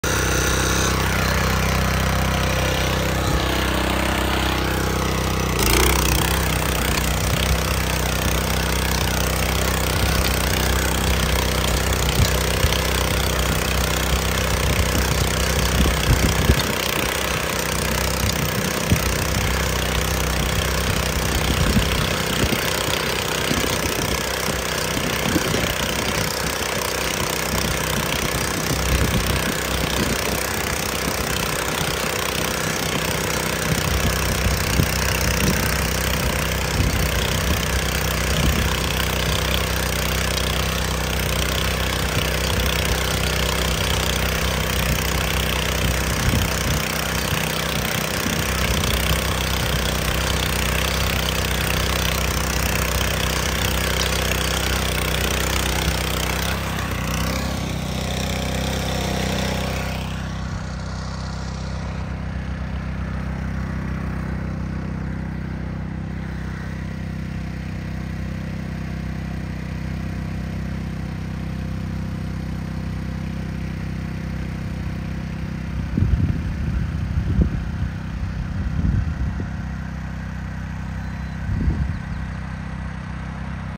Thiessen Tillage wire weeder
Blind weeding sunflowers before they come out of the soil.